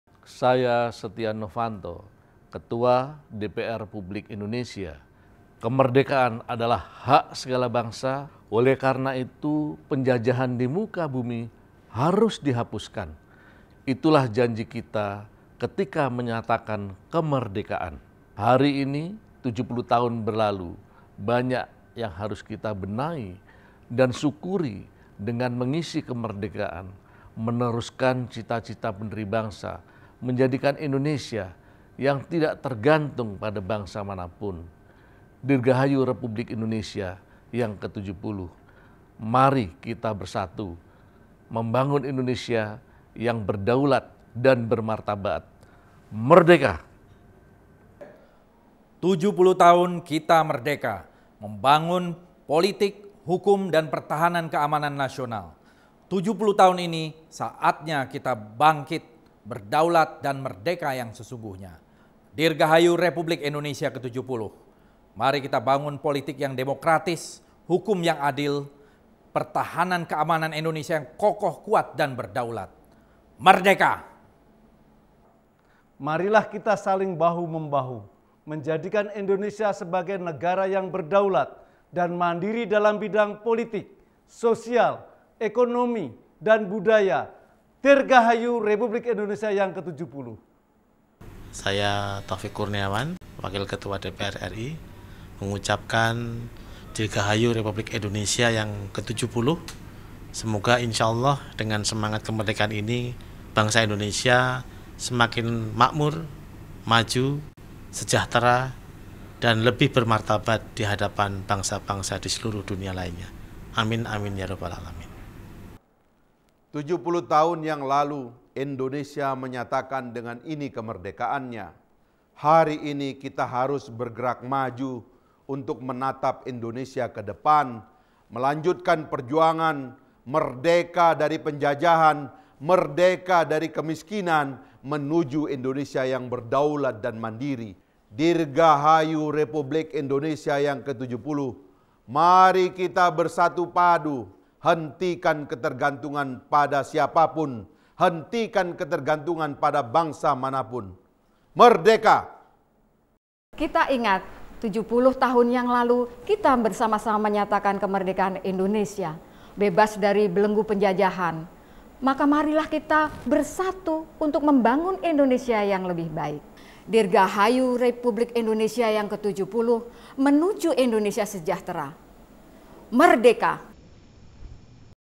DPR RI - "SEMANGAT KEMERDEKAAN" DIRGAHAYU RI - 70
NARSUM : SETYA NOVANTO / Ketua DPR RI / FPG / Dapil NTT II
FADLI ZON / Wakil Ketua DPR RI Bid. KORPOLKAM / FPGERINDRA / Dapil JABAR V
FAHRI HAMZAH / Wakil Ketua DPR RI Bid. KORKESRA / FPKS / Dapil NTB
AGUS HERMANTO / Wakil Ketua DPR RI Bid. KORINBANG / FPD / Dapil JATENG I
TAUFIK KURNIAWAN / Wakil Ketua DPR RI Bid. KOREKKU / FPAN / Dapil JATENG VII
(TVParlemen-Biro Humas & Pemberitaan-Setjen DPR RI)
(Admin:DNS)